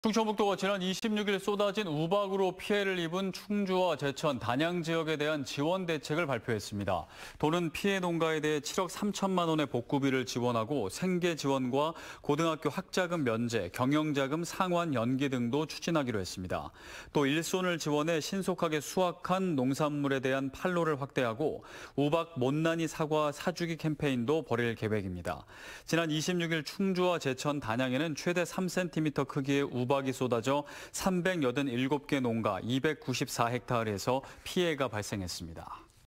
우박 피해지역 지원대책 발표
* 충청북도, 우박 피해농가 복구비 7억3천만 원 지원 *